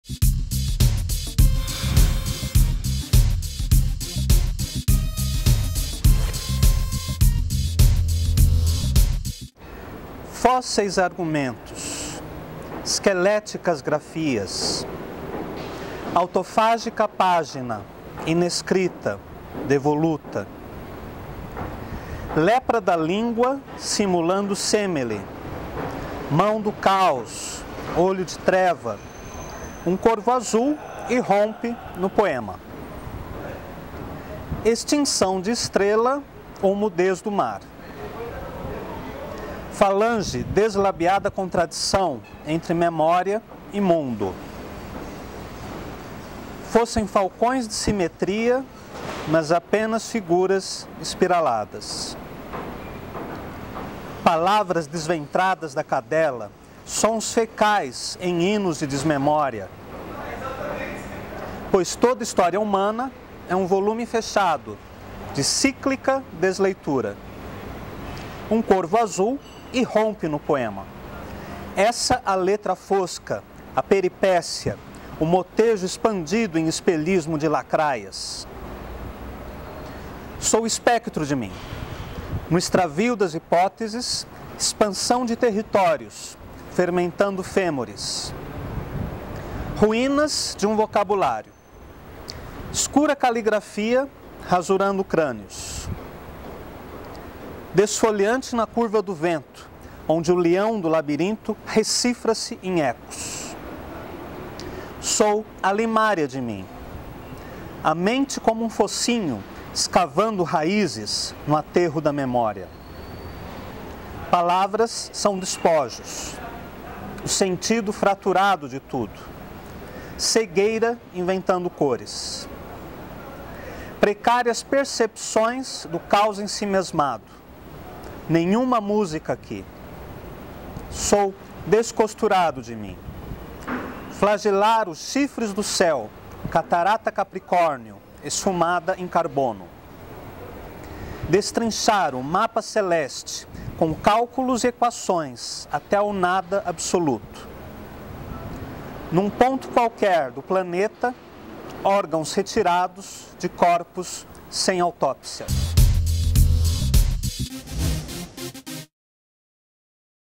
Encontros de Interrogação #10 - Poema de Claudio Daniel
Claudio Daniel apresenta seu poema "Escrito em Osso".

Gravado dia 8 de setembro de 2011, no Itaú Cultural.

Ficha Técnica:
Câmera: Fabricio Noronha
Edição: Anna Lucchese